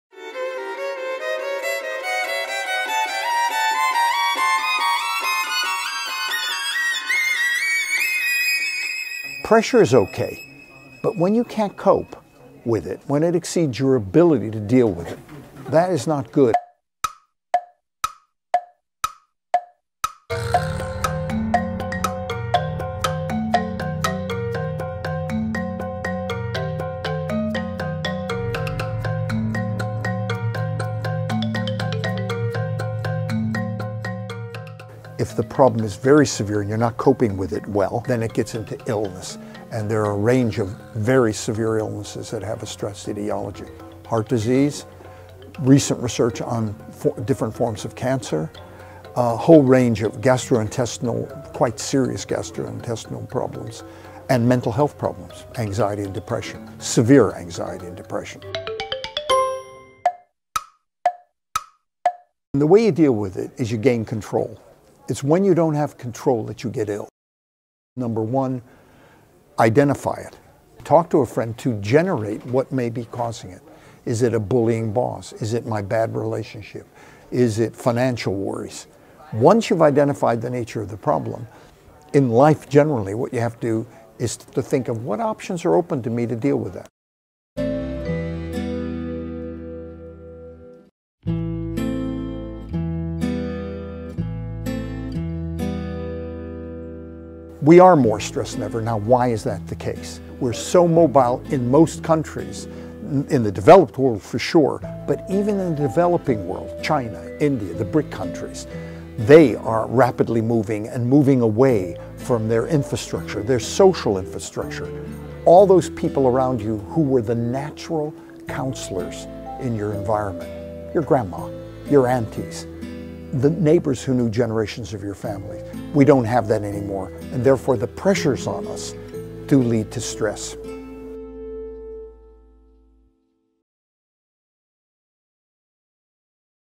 Саломатлик: Стрессдан қандай қутулса бўлади?
Ҳаммамиз ҳаётда стрессга дуч келамиз. Бу ишдан, алоқалар, моддий етишмовчилик ва оиладан ҳам бўлиши мумкин. Биз ҳозир олдинги даврдан кўпроқ стрессга тушяпмиз. Бу нима учун мухим? Стресс бизни касал қилмаслиги учун нима қилишимиз керак?